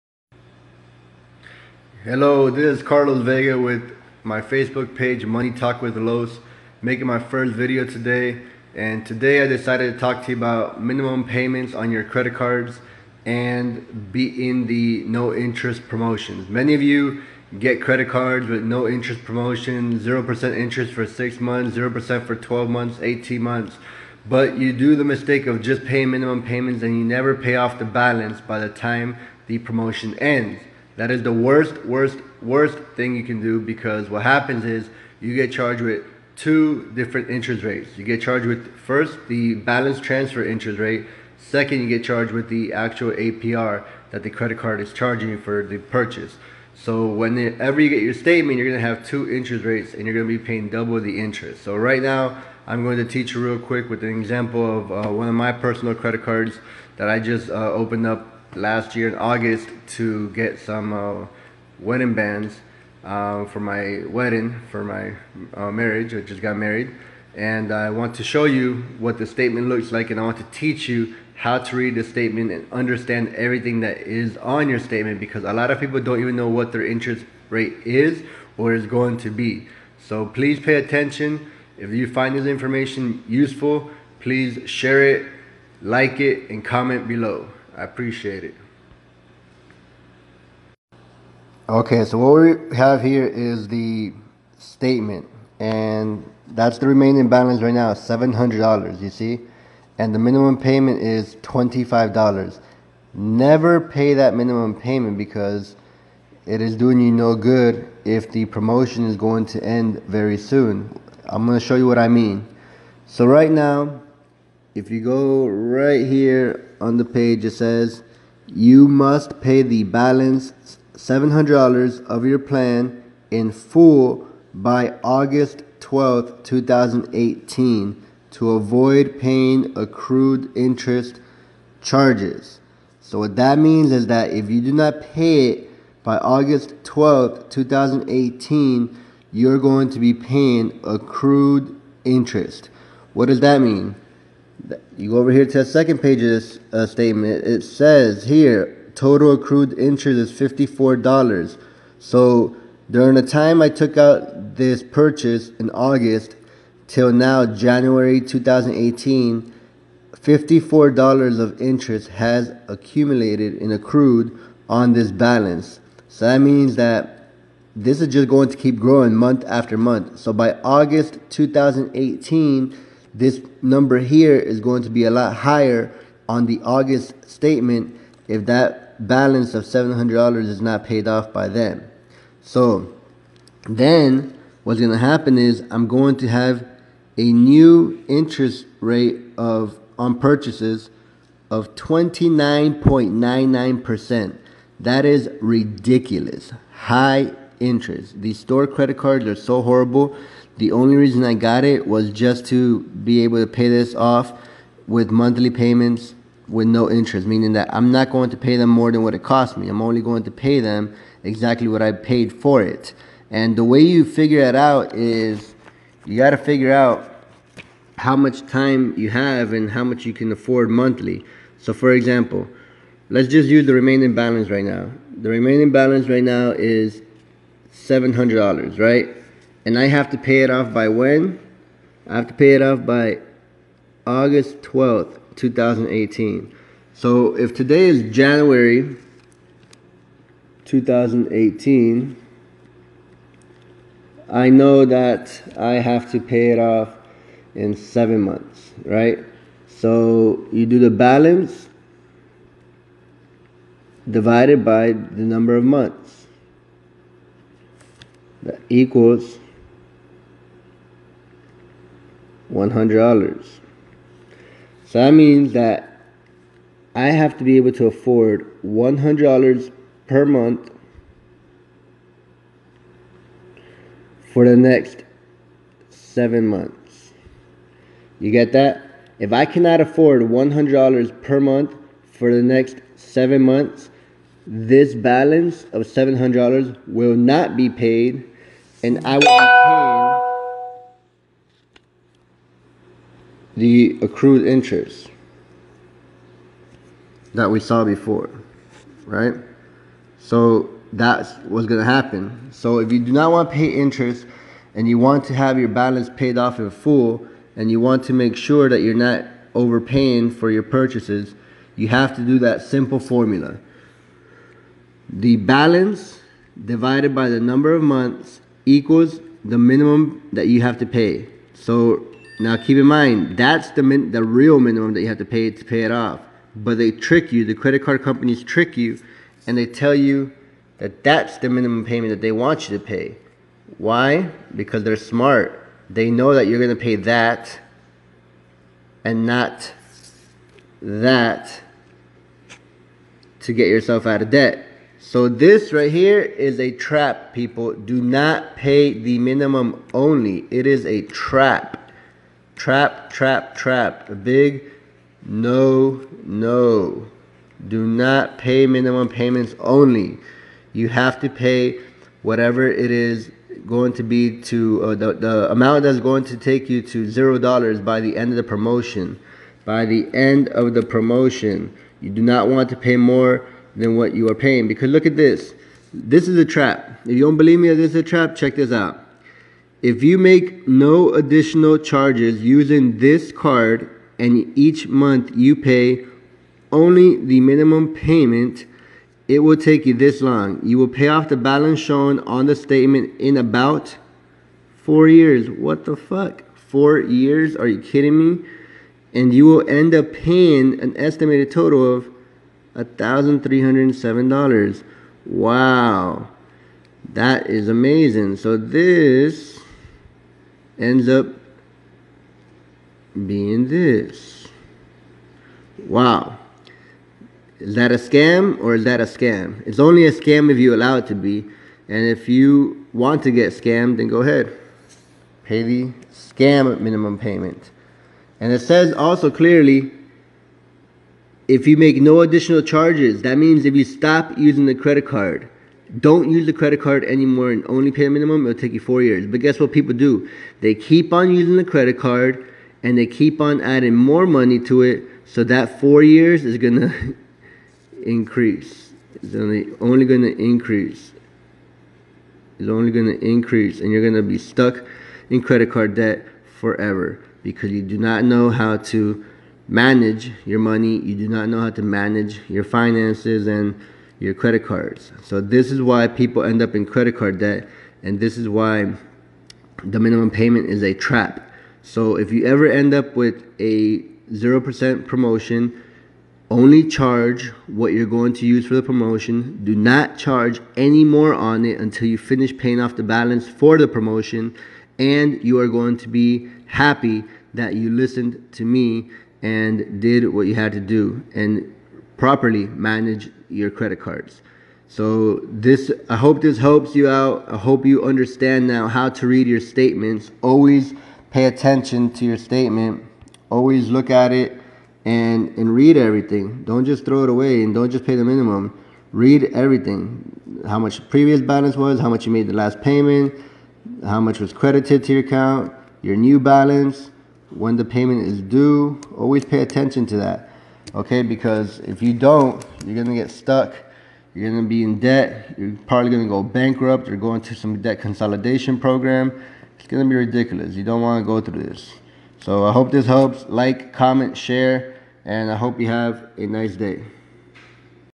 How to not pay interest on credit card promotions MUST SEE
This is a quick video teaching you how to avoid paying interest charges whenever you take out a no interest promotion on a credit card.